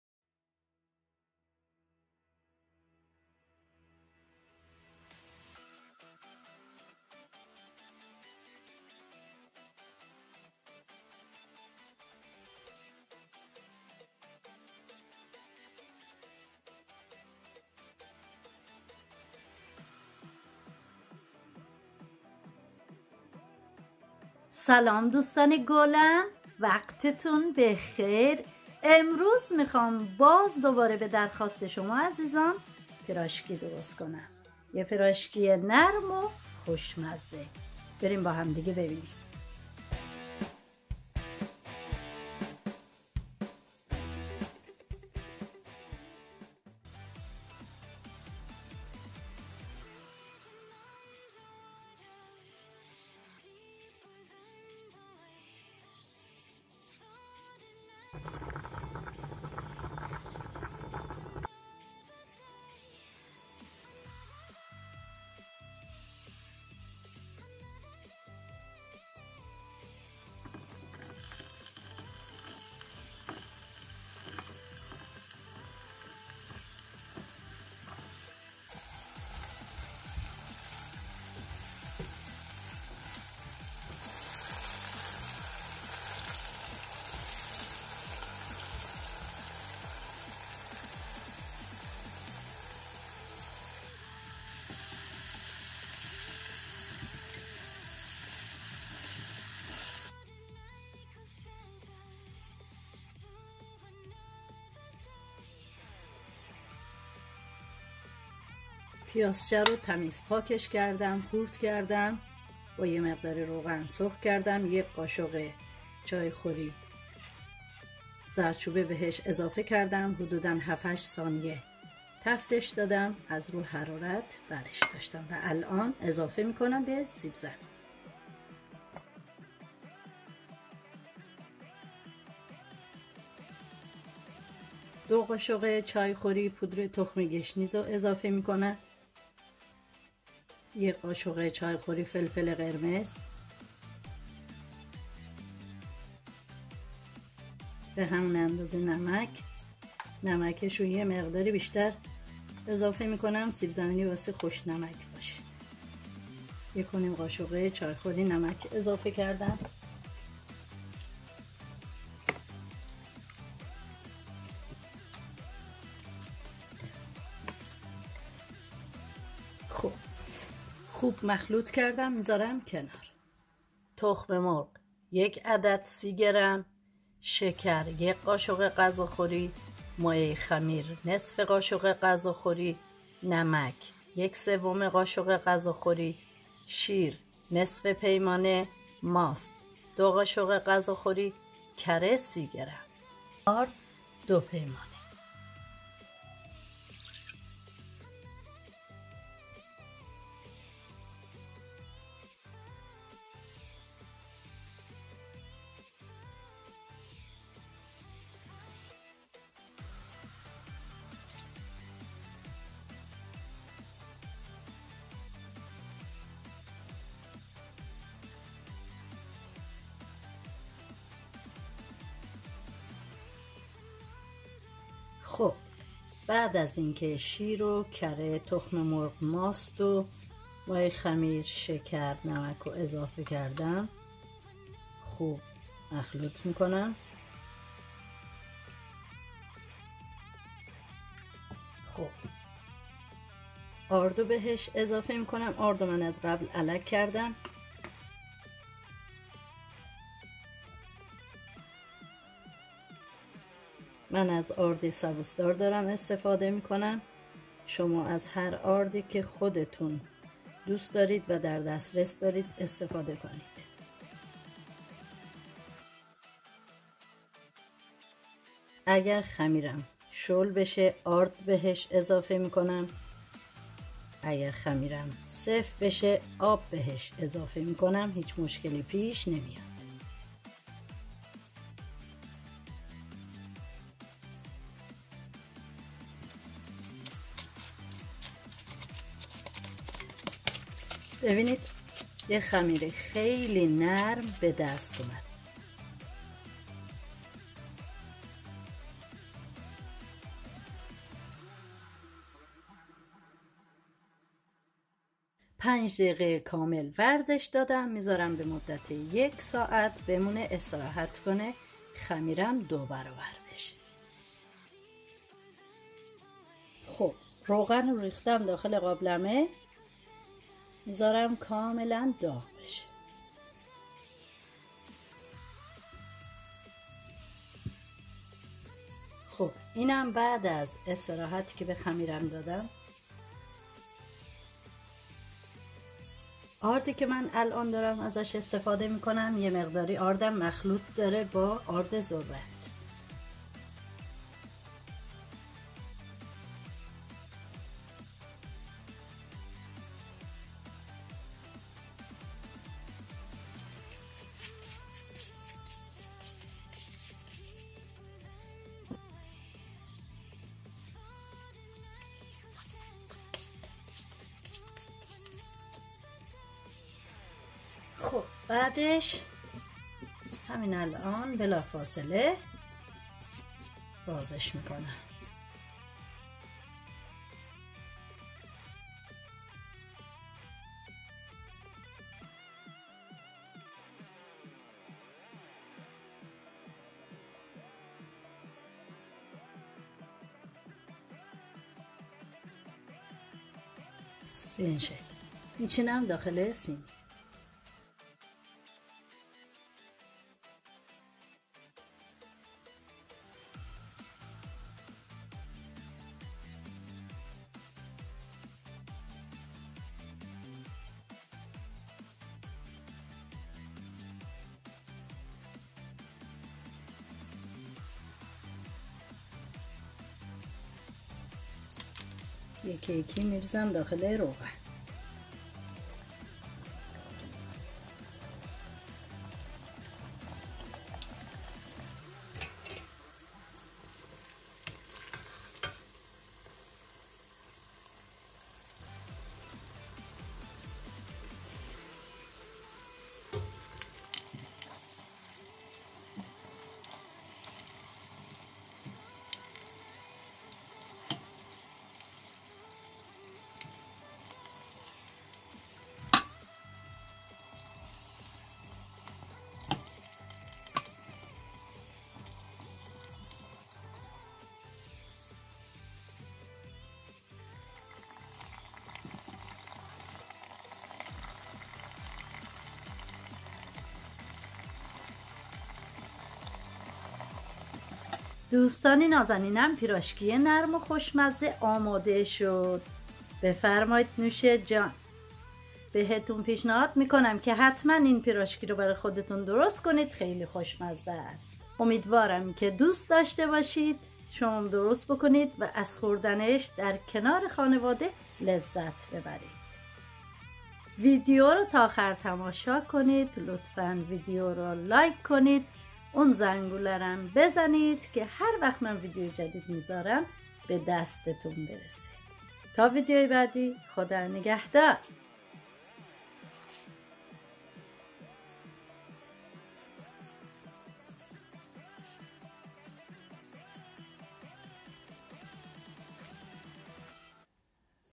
طرز تهیه پیراشکی سیب زمینی، یک غذای فوری، خوشمزه و لذیذ (با مواد کم) - Persian Food
۴۰۰ گرم ارد
۳۰ گرم کره
۳۰ م ل شیر
۲ ق.غ. شکر
۱/۲ ق.غ. مایه خمیر
۱/۳ ق.غ. نمک
۱ تخم مرغ
نیم کیلو سیب زمینی
یک دسته پیازچه
۱ ق.غ. زردچوبه
۱ و نیم ق.چ. نمک
۱ ق.چ. فلفل قرمز
۲ ق.چ. پودر تخم گشنیز

400 grams fluor
30 grams butter
100 ml milk
2 tablespoons suger
½ tablespoon yeast
⅓ tablespoon salt
1 egg
½ kg potatoes
A bunch of scallion
1 teaspoon turmeric
1 + ½ teaspoons salt
1 teaspoon red peppar
2 teaspoons coriander seeds